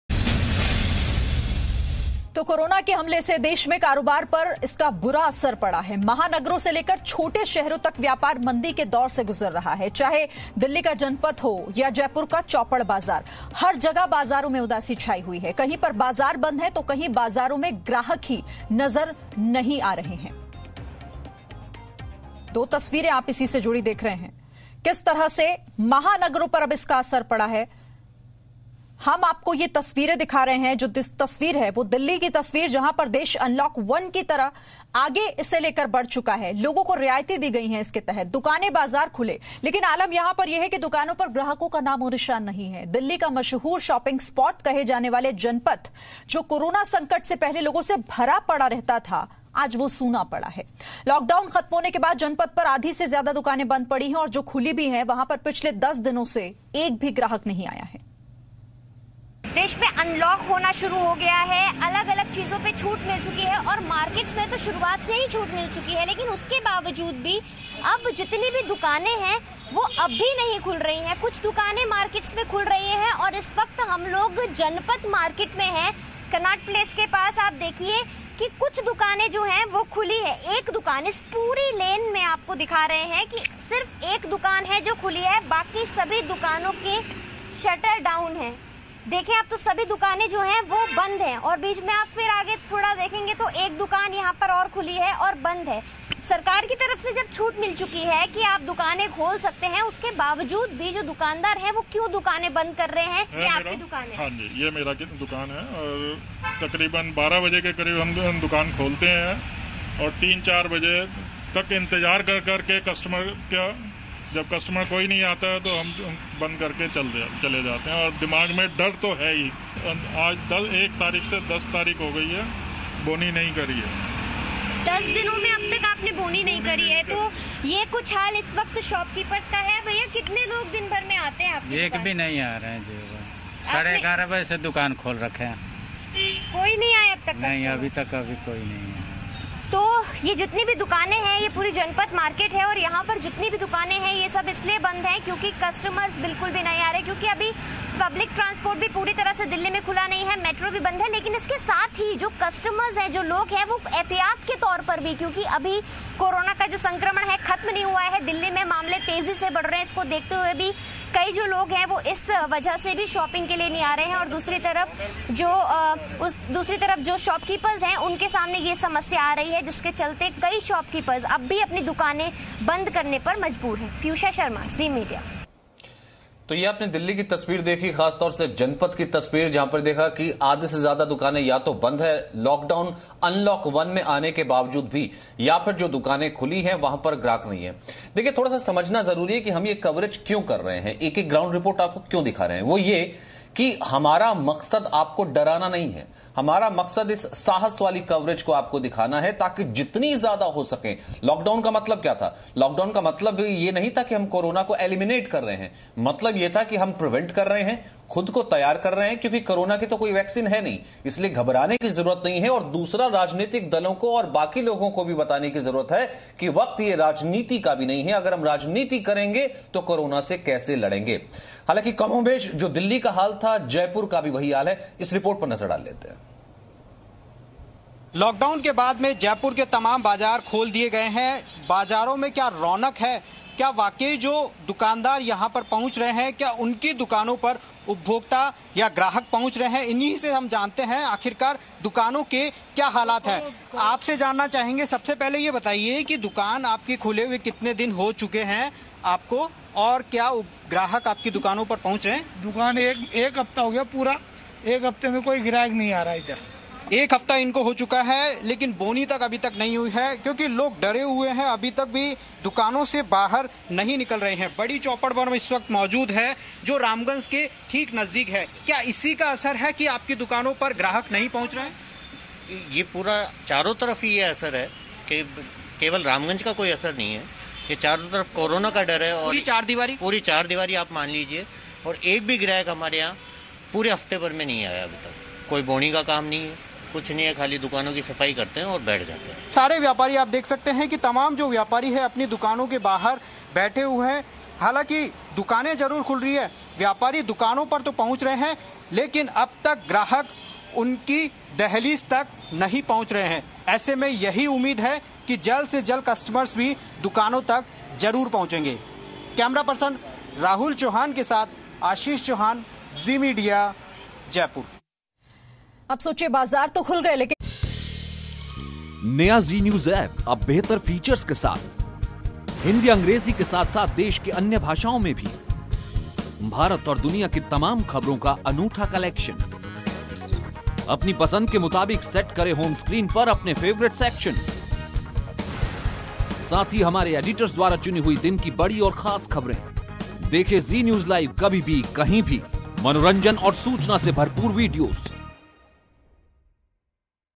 Corona की दूसरी लहर से डर में Delhi, सूने पड़े बाजार । Ground Report from Delhi Market
Delhi is on high alert from the second wave of Coronavirus. Watch this Ground Report of Delhi's Famous market which are almost blank despite governments order

दिल्ली में कोरोनावायरस की दूसरी लहर ने आतंक फैला दिया है। सरकार ने बाजार खोलने की मंजूरी दे दी है लेकिन अब भी दिल्ली के बड़े फेमस बाजार बंद पड़े हैं। दुकानों में लोगों की भीड़ नहीं है। कारोबार बिल्कुल ठप है। देखिए ये ग्राउंड रिपोर्ट